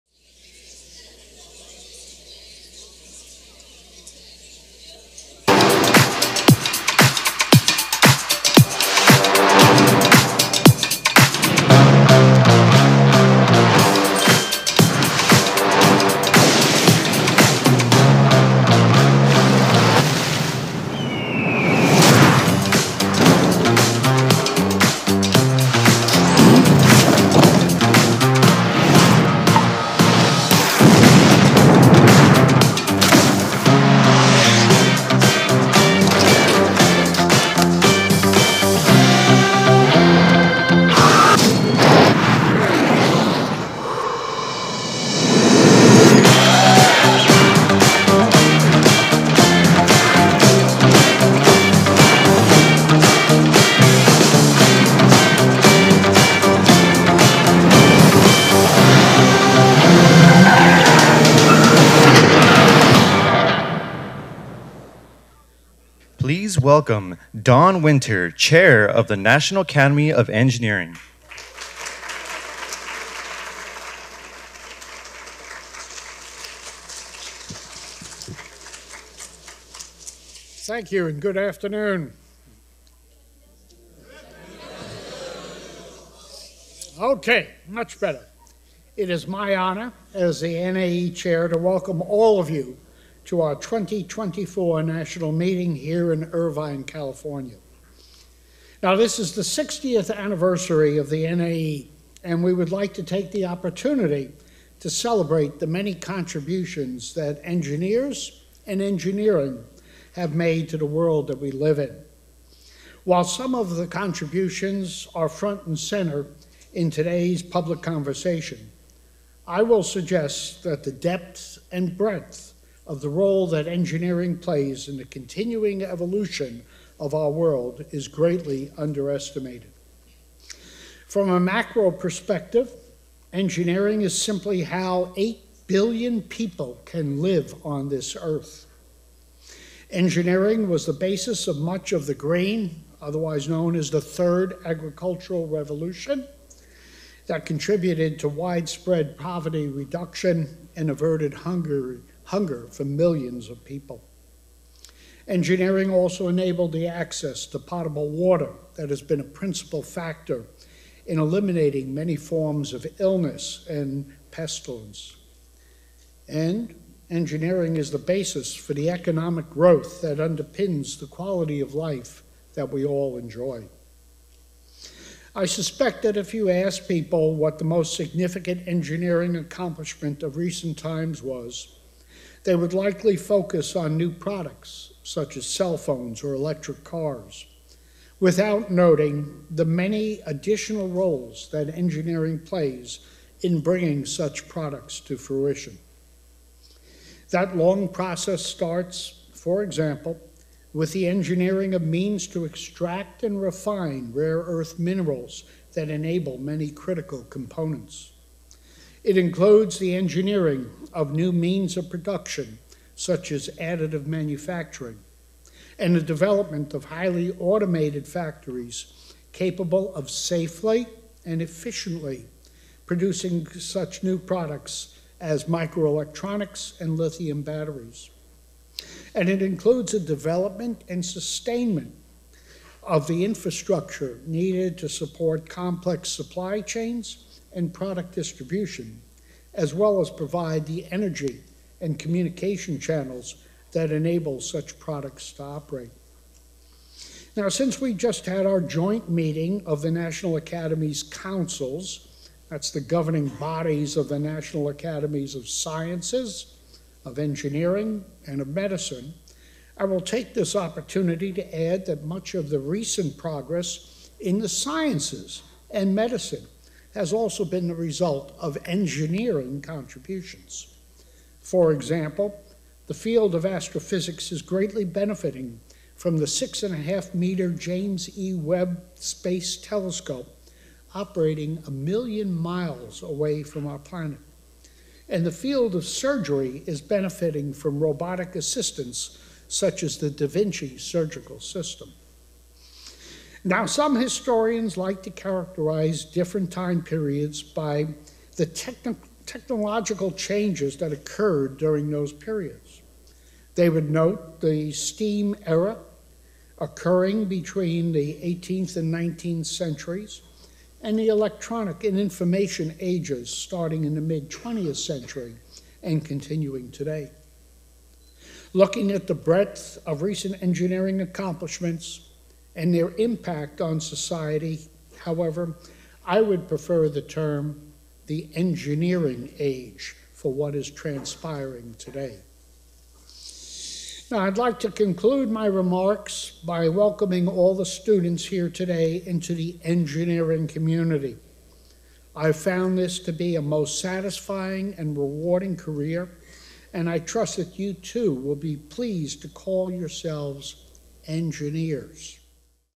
Don Winter, NAE Chair, Opening Remarks: 2024 NAE National Meeting
NAE Chair Don Winter delivers opening remarks at the 2024 NAE National Meeting.